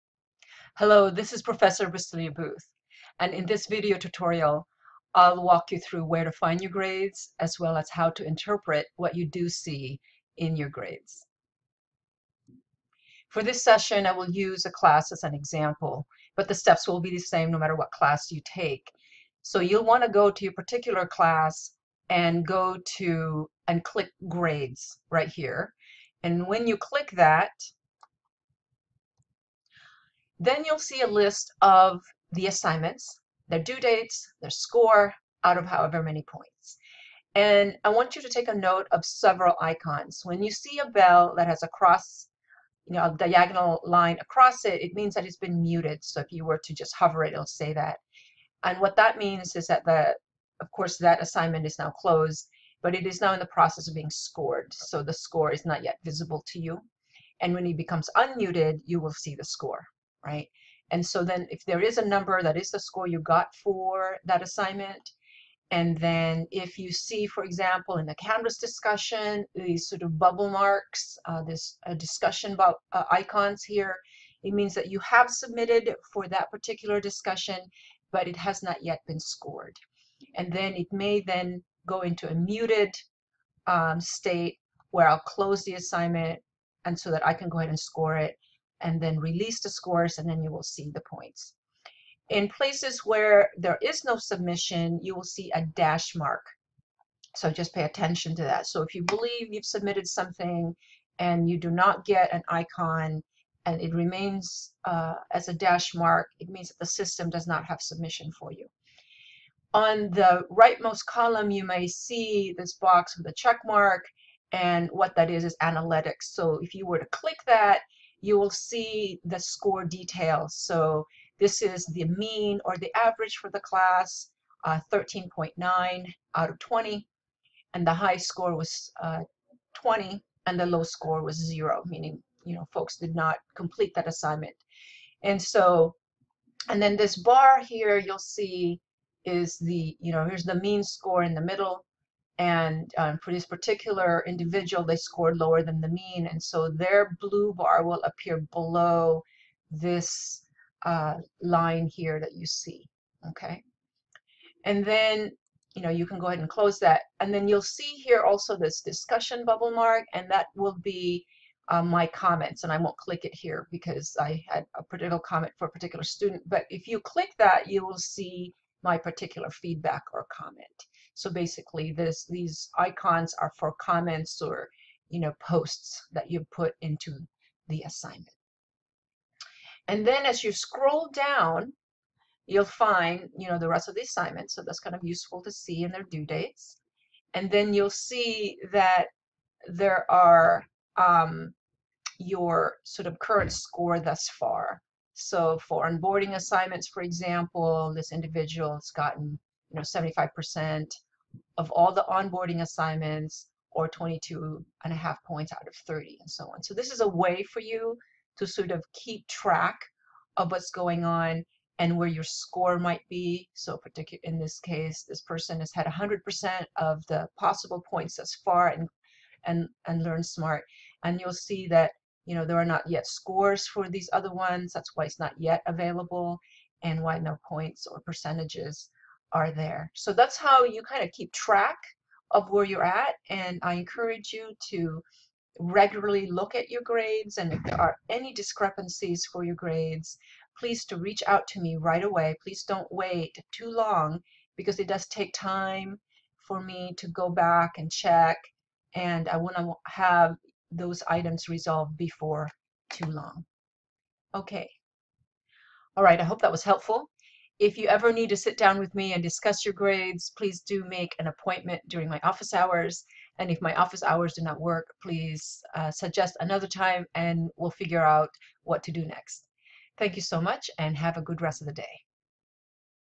How to Check Grades (and Submissions) in Canvas
Prof. Bustillo-Booth demonstrates how to find grades and check submissions in Canvas.